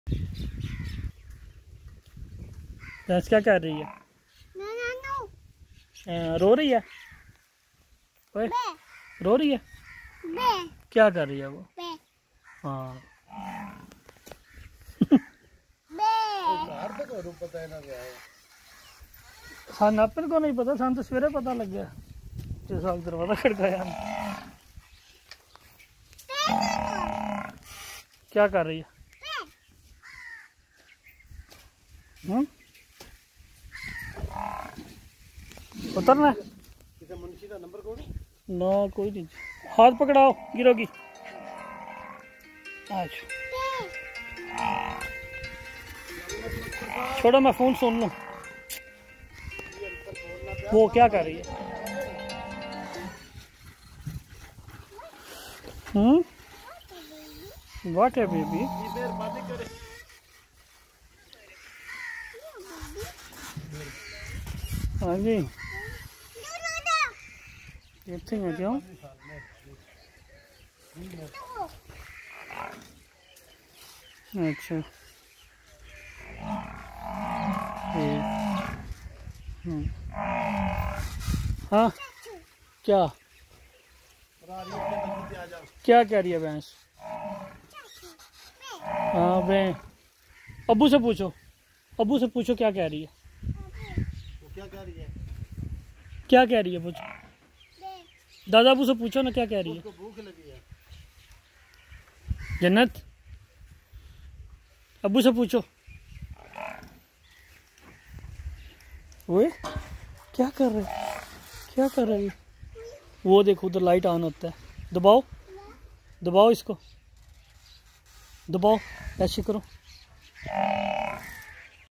cute baby in village
my beloved jannat Fatima first time in village